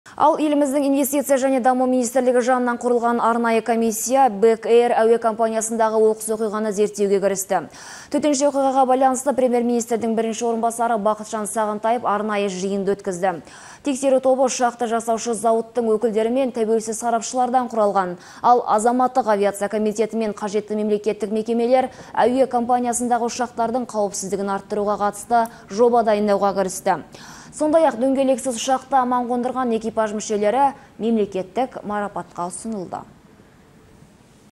Арнайы комиссия "Бек Эйр" ұшағына қатысты істі зерттеуге кірісті (29.03.16)
Төтенше оқиғаға байланысты Премьер-министрдің бірінші орынбасары Бақытжан Сағынтаев арнайы жиын да өткізді.